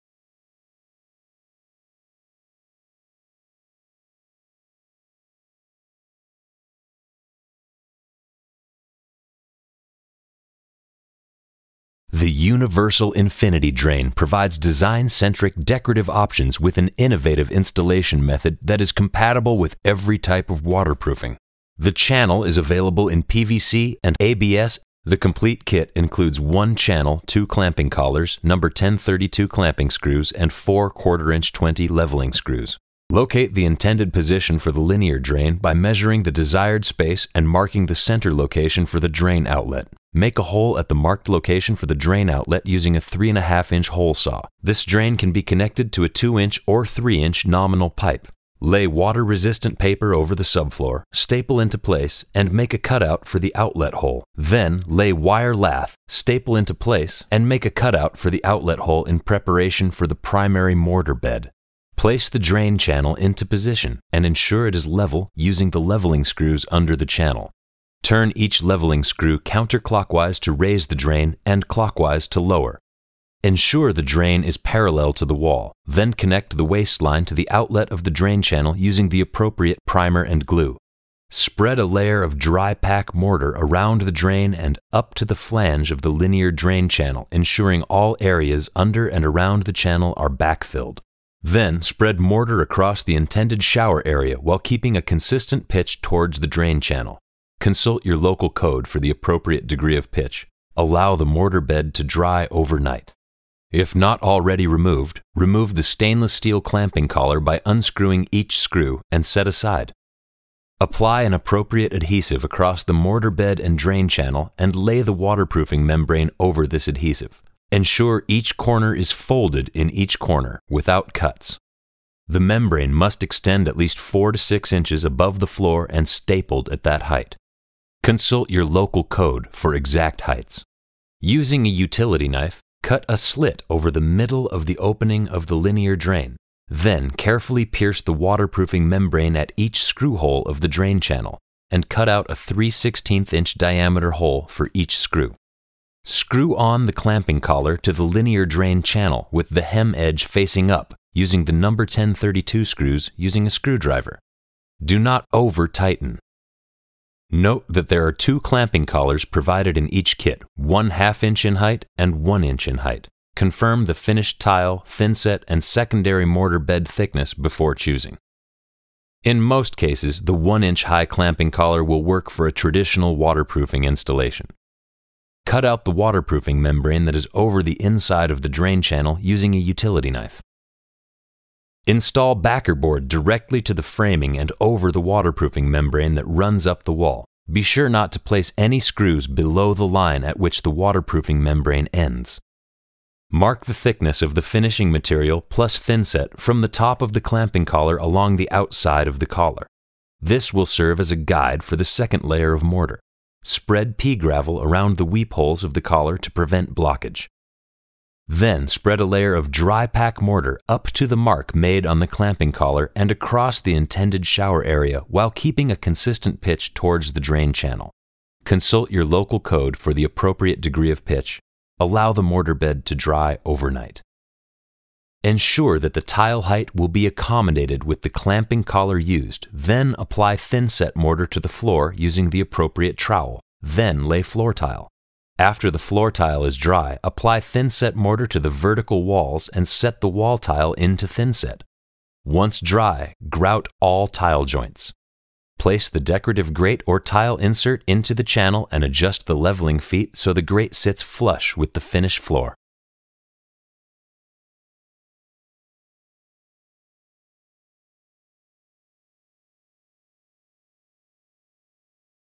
Universal Infinity Drain Linear Drain Installation Video - Traditional Waterproofing
Infinity Drain, the manufacturer at the forefront of architectural and decorative drains for both indoor and outdoor applications, continues to simplify barrier free bathroom design with its new Universal Infinity Drain® linear drain that is compatible with most types of waterproofing.  The convenient universal kit features a pitched channel with a clamping collar and the choice of a tile insert frame or squares pattern grate.  This installation video is specific to vinyl pan / PVC shower pan liner / Chloraloy waterproofing methods for tiled shower enclosures.